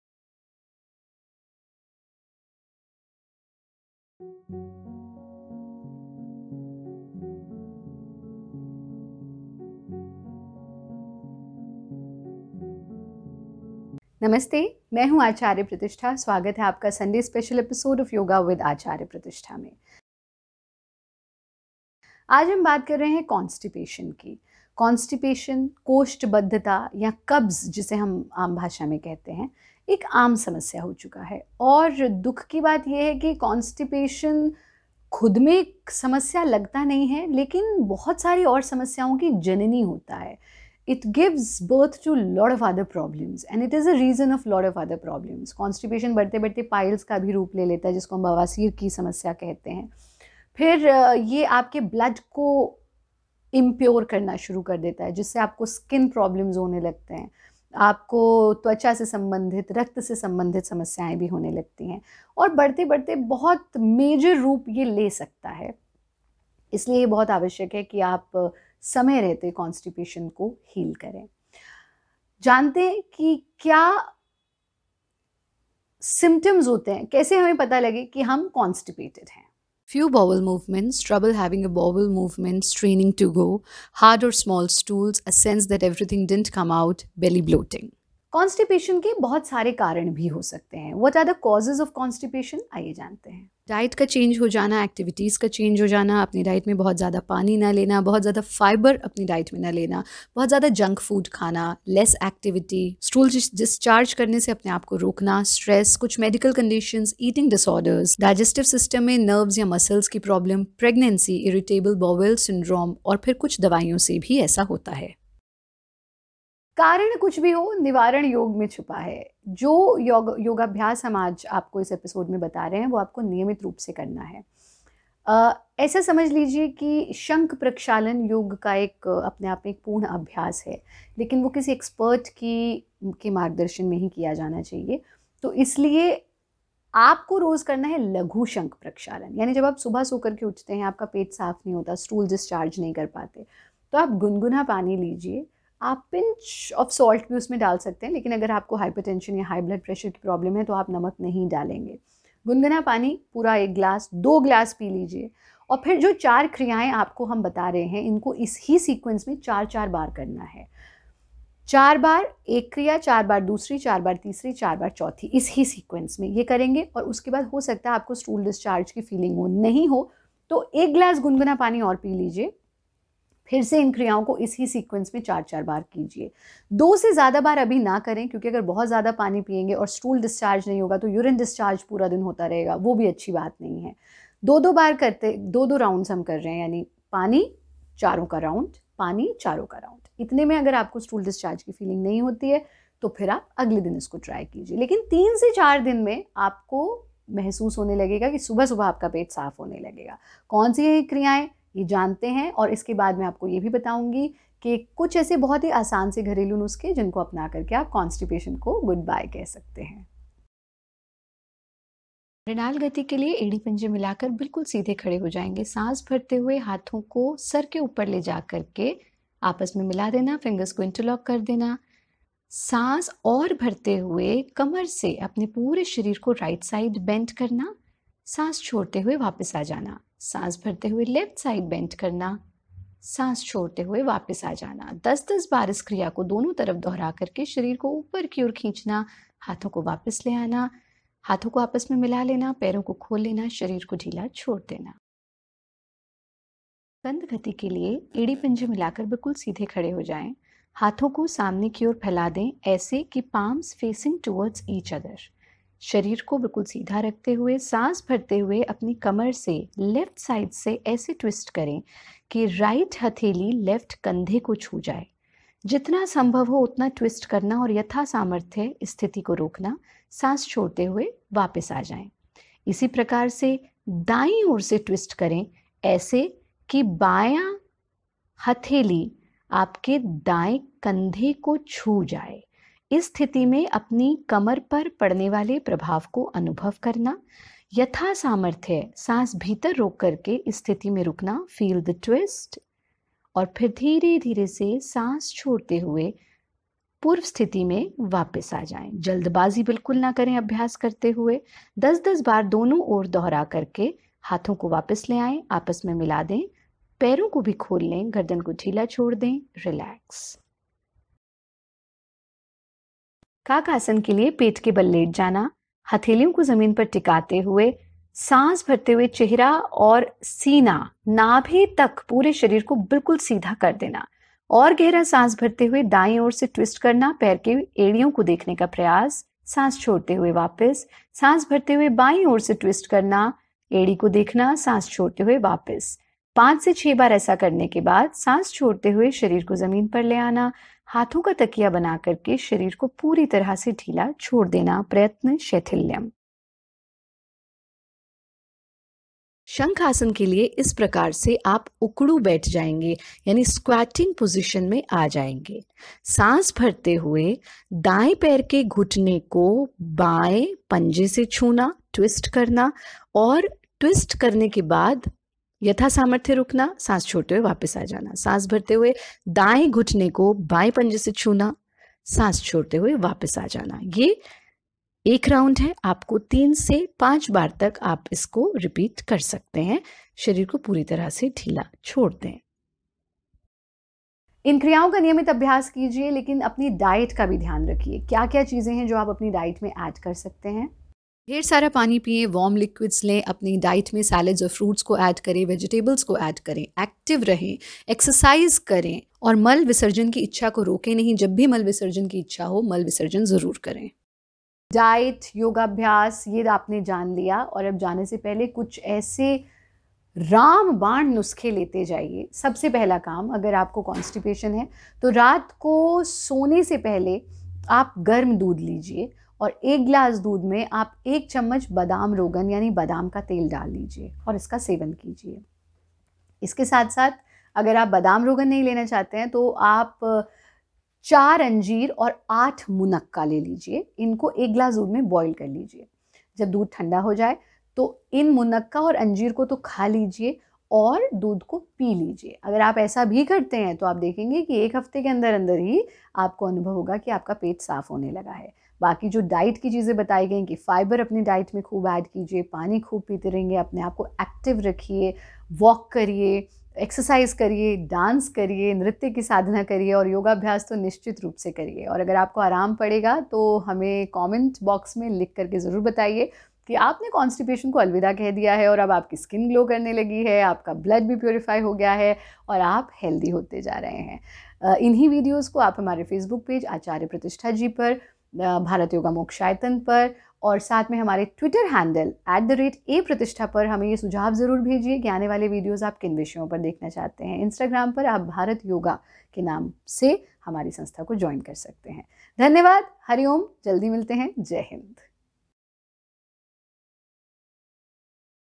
पेट साफ़ व् कब्ज़ को जड़ से खत्म करने का अचूक इलाज |Full Yoga, Diet&Home Remedies to cure Constipation.
Acharya Pratishtha ji shares complete Yoga schedule, diet and home remedies to cure constipation completely.